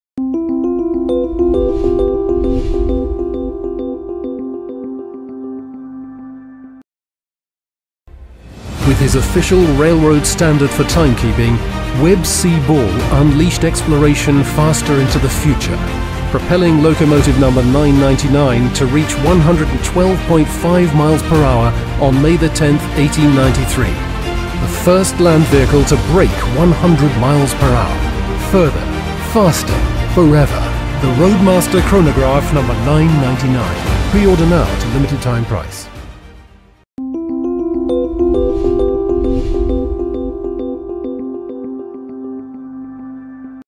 BALL Watch Company - NEW Roadmaster Chonograph No. 999 l Jura Watches
The Roadmaster Chronograph No. 999 honors the first land vehicle to break 100 mph, timed by our founder Webb C. Ball on May 10, 1893. Gear up with tachymeter functionality, COSC precision, 42 mm titanium case and micro gas lights.

Coming soon to Jura Watches!